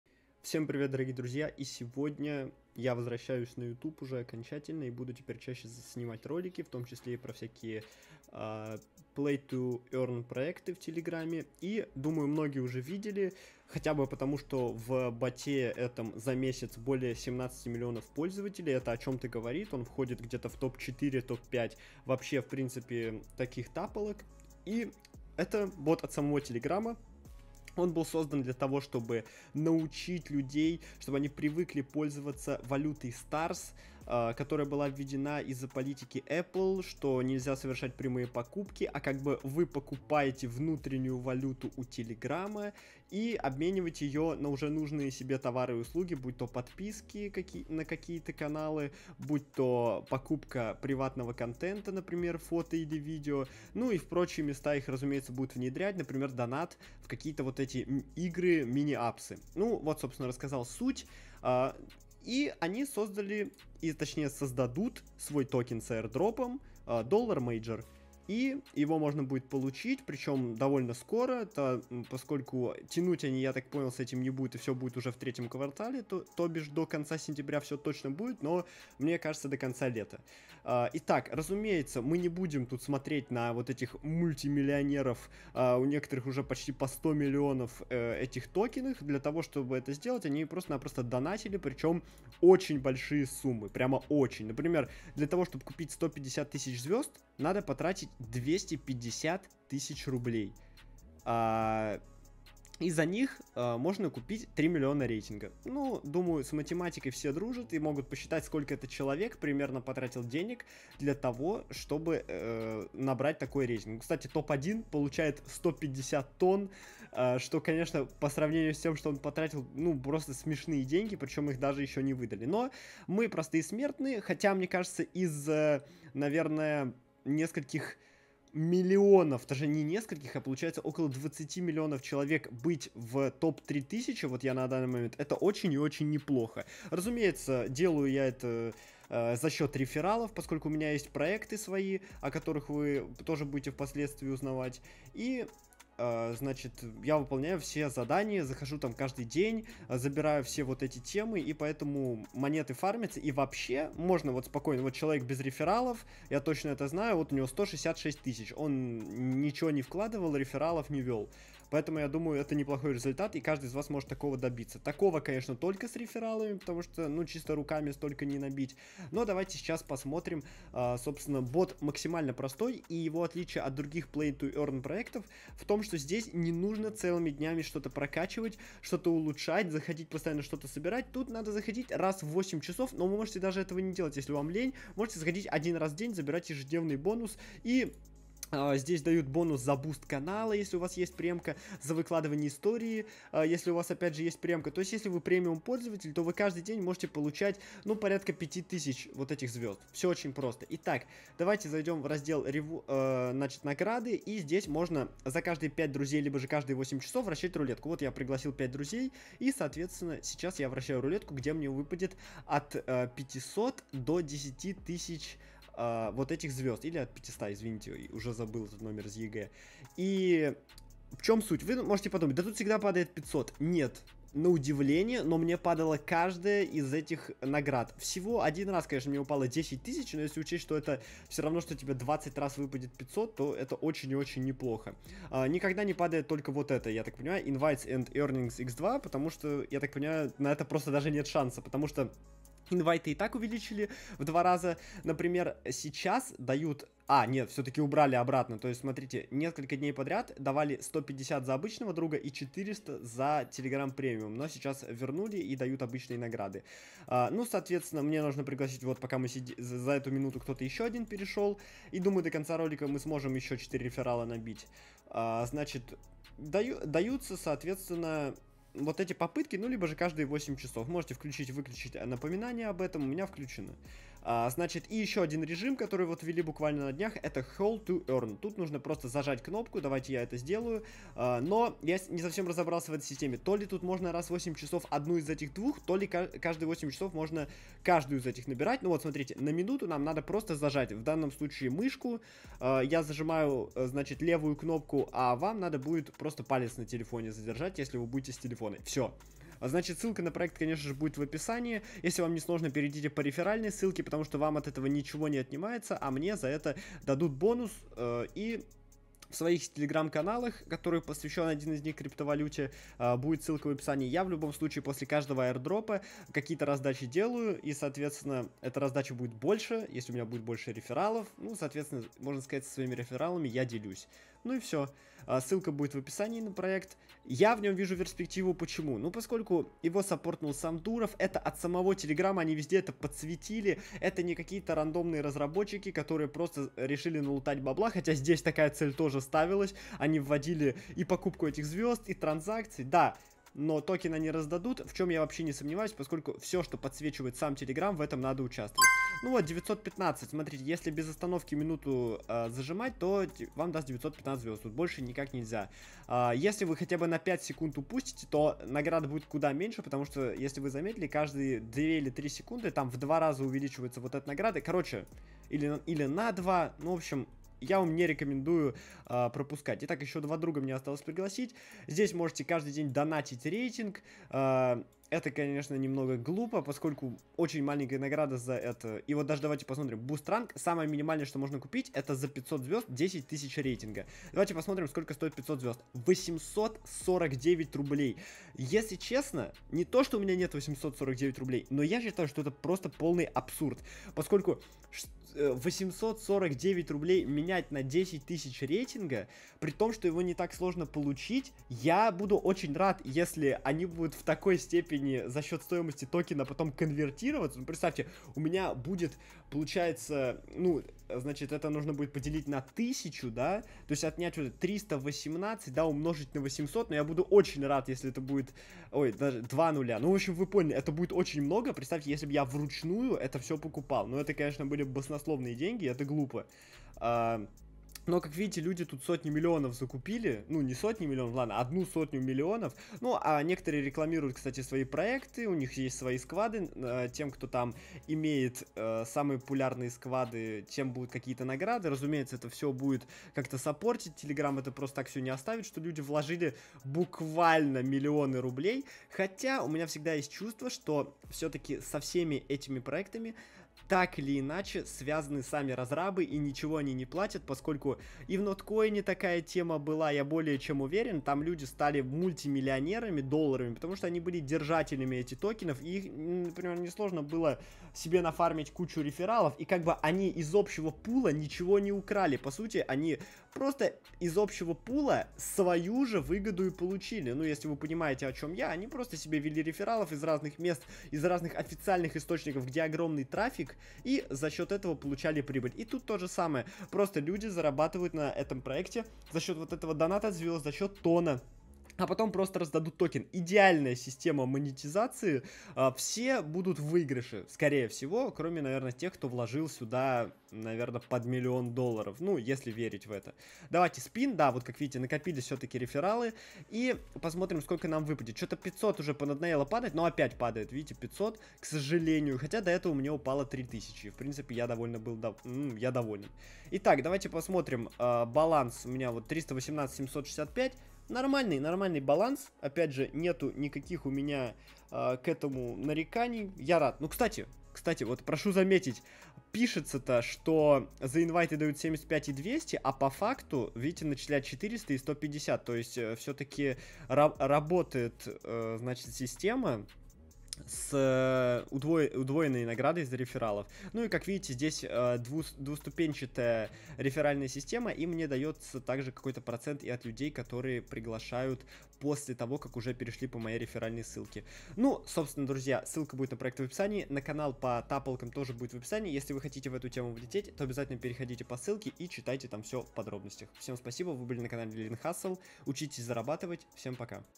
MAJOR — мощный AIRDROP от Telegram / P2E #1
В сегодняшнем видео я расскажу вам про проект Major, который запустил сам Telegram и всячески просаппортил. В скором времени будет объявлено об аирдропе токена $MAJOR, который сможет заполучить каждый из вас, просто перейдя по ссылке выше.